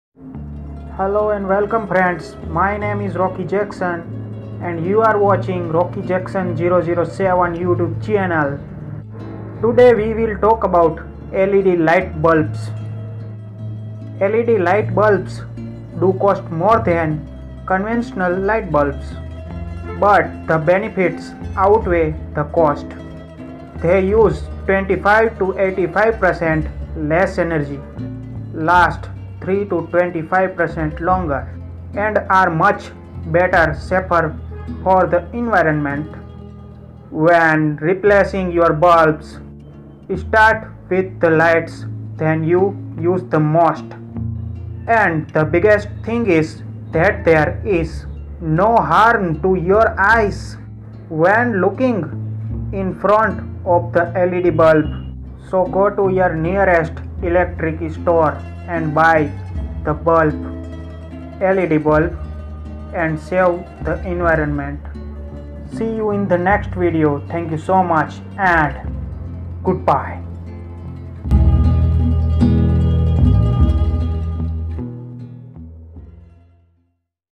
Use LED Light Bulbs and Save Power - Save Environment | Rocky Jackson 007
Hello friends 👋
Welcome to The Rock Family 🎸
My name is Rocky Jackson ❤️
And you are watching Rocky Jackson 007 YouTube channel 😀

Today we will talk about LED light bulbs. LED light bulbs do cost more than conventional light bulbs, but the benefits outweigh the cost.They use 25-85% less energy, last 3-25% longer, and are muchbetter/safer for the environment.

Language:- English